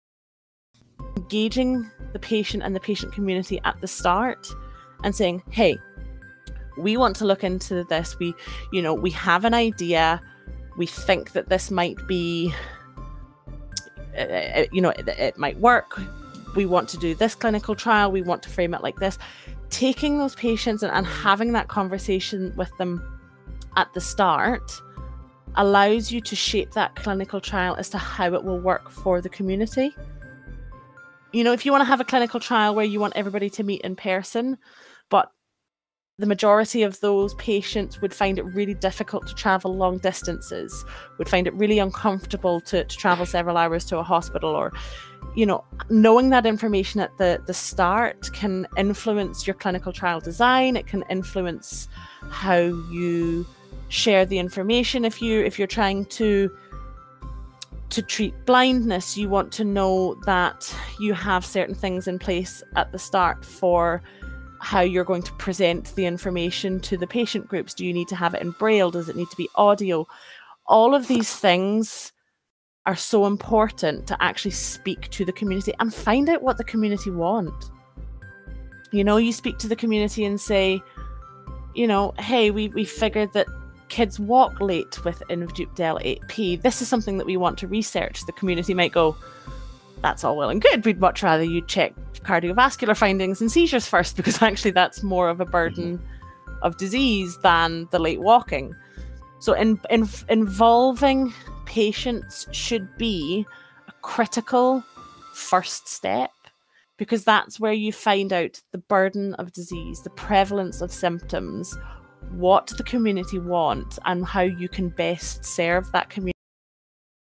Benefits of Patient Involvement in Drug Development | Real Talk, Real World Data Highlights
Patients are a critical part of the drug development process. However, they are often not involved in shaping clinical trials and studies and are contacted when it is time to recruit. This often results in a trial design that does not work for the patient community. Weather it is not factoring the burden of travel, or focusing on the wrong burden of disease, these problems can be overcome by involving patients and their caregivers at the very beginning of the drug development process. Listen to our first guest, Becky Pender share her thoughts on this issue.